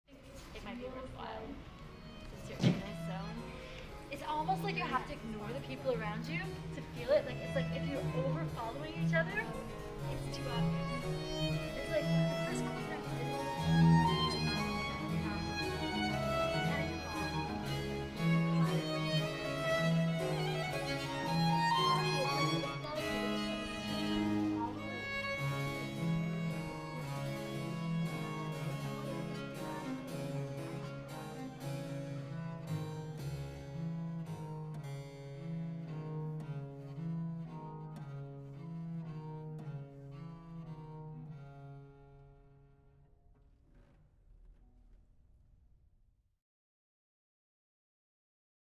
Lean-to - Lesley Telford (Rehearsal, Ballet BC, 2021)
In October 2021 Lesley Telford joined Ballet BC in studio to create her latest work Lean-to.
Artists: Artists of Ballet BC, Lesley Telford
Video and Editing: Peter Smida
Composed by: Marin Marais
Performed by: Early Music Vancouver
- Chloe Meyers, violin
- Natalie Mackie, viola da gamba
- Alexander Weimann, harpsichord
Audio recording producer: Grant Rowledge
Audio recording venue: Pacific Spirit Church, October 16 2021